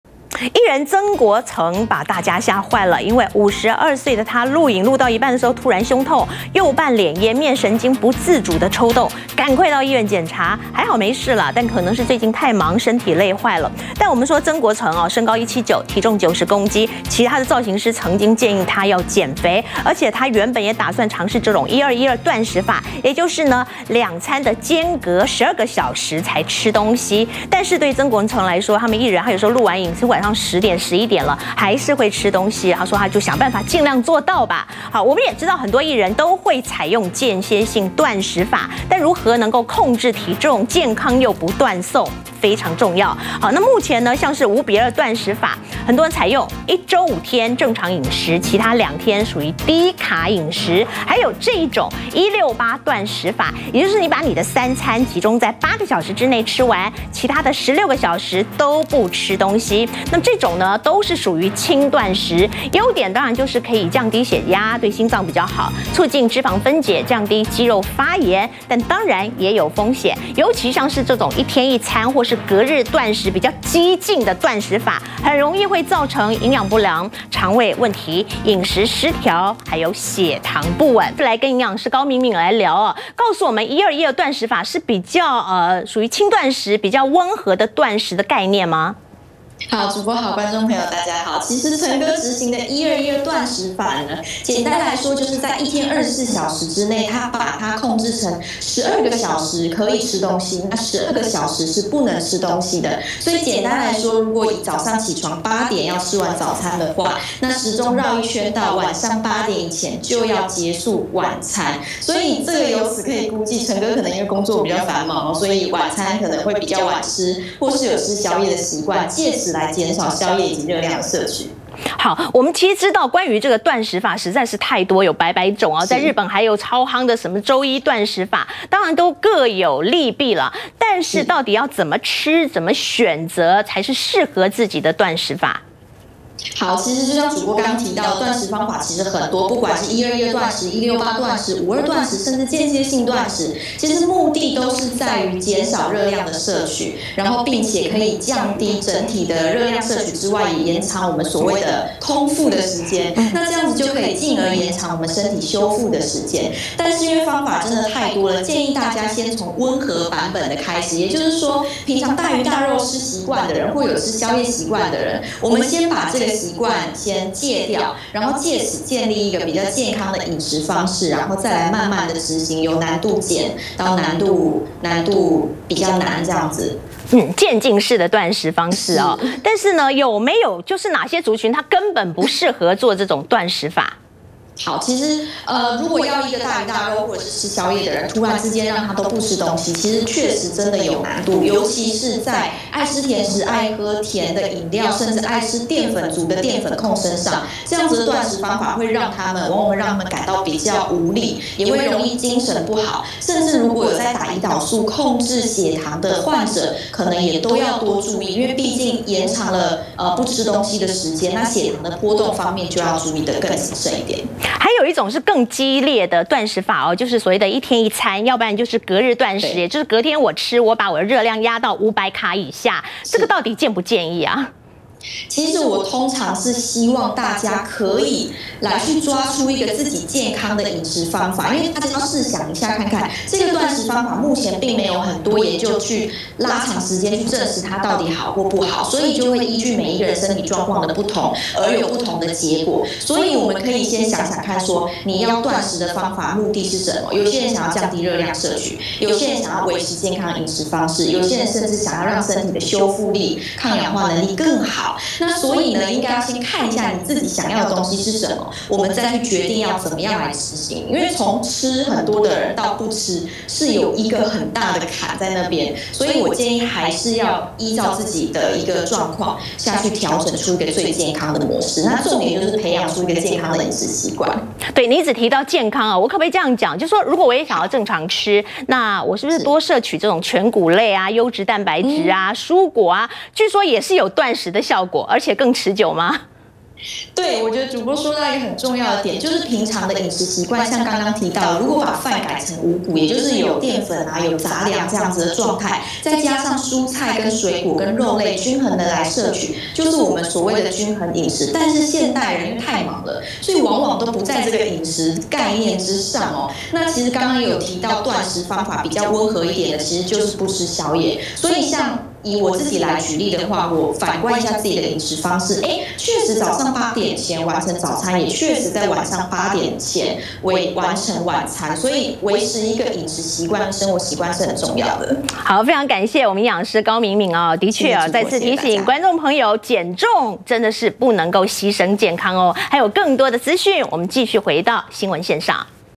HOT新聞開箱》減重「間歇式斷食」超有效？各種斷食法風險先了解 營養師提醒「這些族群」不適合！｜2020.07.17
受訪者：營養師 高敏敏